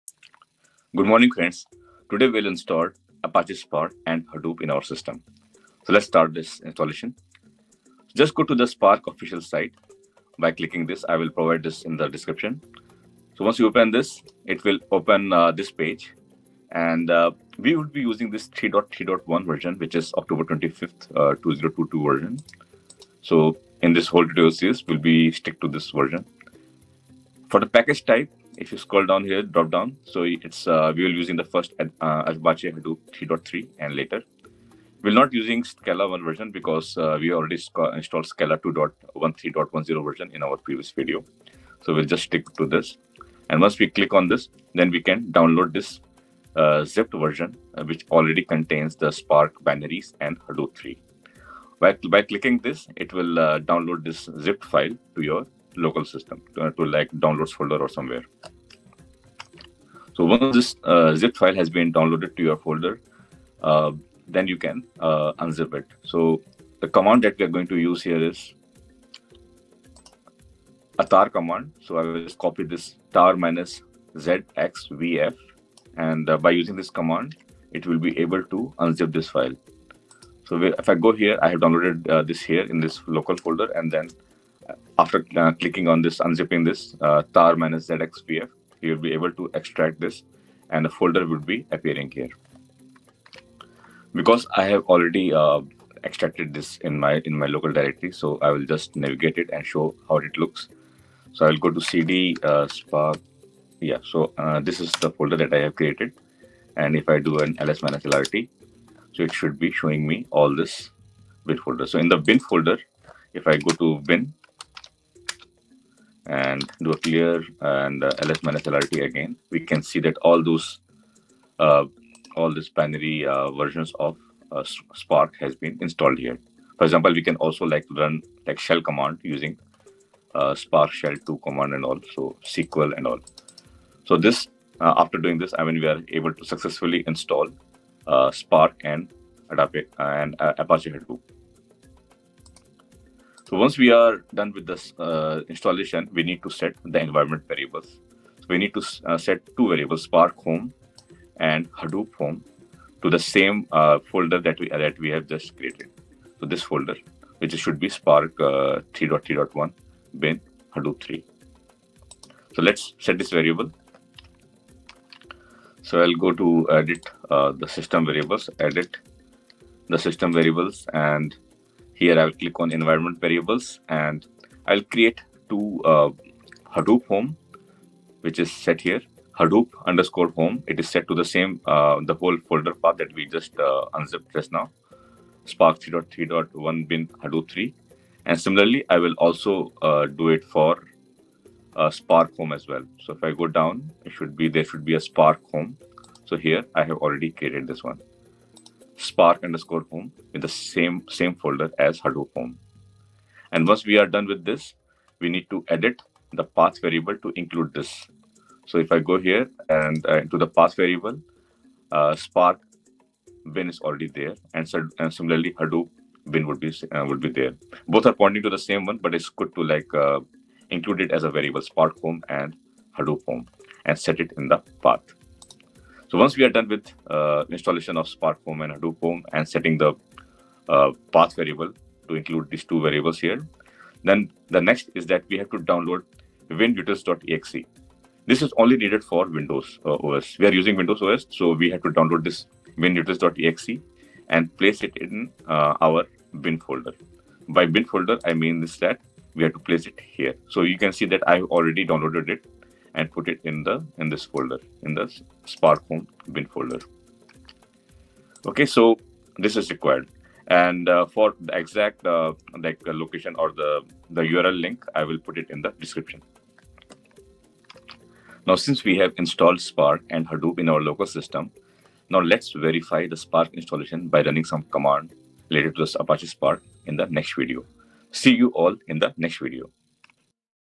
12 - Apache Spark and Hadoop installation
Chapter 02 - Apache Spark and Hadoop installation

3) Unzip the file to local folder: tar -zxvf spark-3.3.1-bin-hadoop3.tgz

4) Set SPARK_HOME environment variable to |downloaded folder|\spark-3.3.1-bin-hadoop3

5) Set HADOOP_HOME environment variable to |downloaded folder|\spark-3.3.1-bin-hadoop3

6) Add to the PATH environment, the %SPARK_HOME%\bin

7) Add to the PATH environment, the %HADOOP_HOME%\bin

9) Place both the files in local %SPARK_HOME%\bin folder.